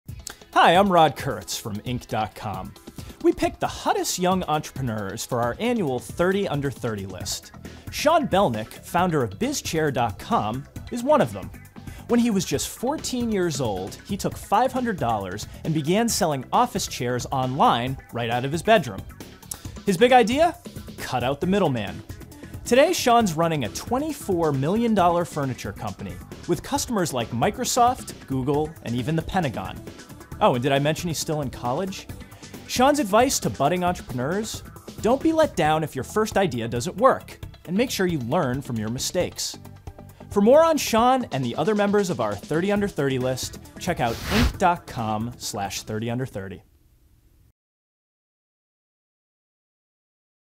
Inc Magazine's 30 Under 30: Sean Belnick of Bizchair.com
Entrepreneur Sean Belnick of Bizchair.com.